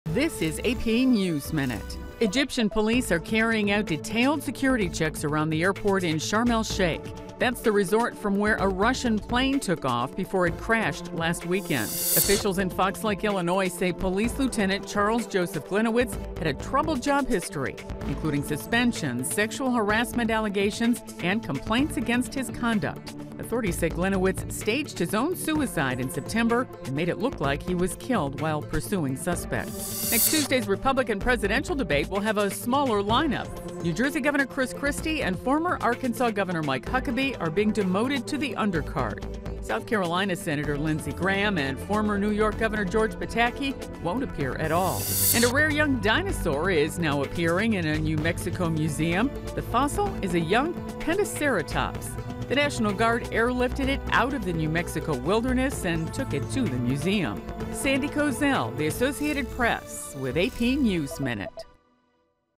AP Top Stories 6 A
Here's the latest for Friday, November 6th: Security tight at Egyptian airport; Illinois police officer's record scrutinized; GOP debate field shrinking; Young dinosaur on display.